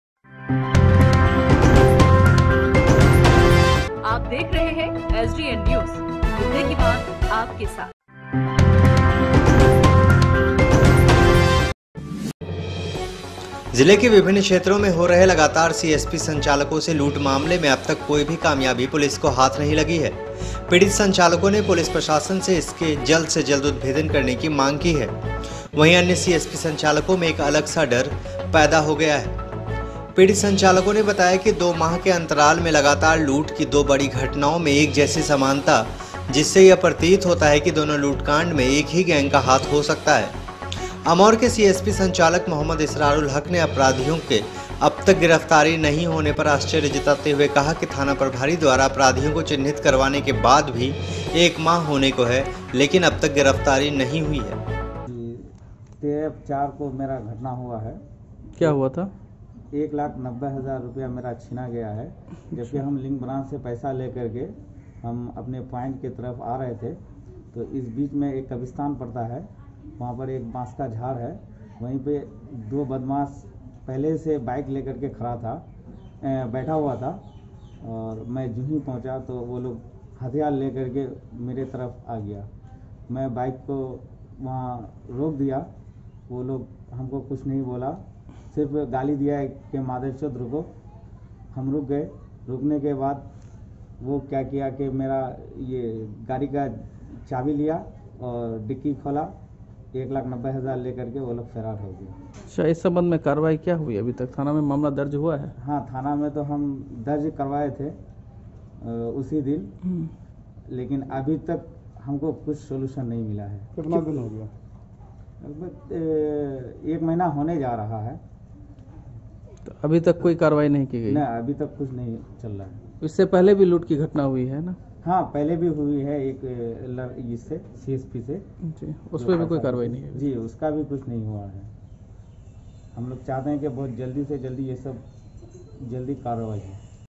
लगातार सीएसपी संचालकों से लूट मामले में अब तक कोई कामयाबी पुलिस को नहीं मिली है - संचालको ने की मांग
जिले के विभिन्न क्षेत्रों में हो रहे लगातार सीएसपी संचालकों से लूट मामले में अब तक कोई कामयाबी पुलिस को नहीं मिली है | पीड़ित संचालको ने पुलिस प्रशासन से इसके जल्द से जल्द उदभेदन करने की मांग की है | वही अन्य सीएसपी संचालको में एक अलग तरह के डर का माहौल बढ़ता जा रहा है | पीड़ित संचालको ने बताया कि  दो माह के अन्तराल में लगातार लूट की दो  घटनाओं में एक जैसी समानता जिससे यह प्रतीत होता है कि दोनों लूटकांड में एक ही गैंग का हाथ हो सकता है | अमौर के सिएसपी संचालक मोहम्मद इसरारूल हक ने अपराधियों के अब तक गिरफ्तारी नहीं होने पर आश्चर्य जताते हुए कहा की थाना प्रभारी द्वारा अपराधियों को चिन्हित करवाने के बाद भी एक माह होने को हो गए हैं .लेकिन अब तक गिरफ्तारी नहीं हुई है |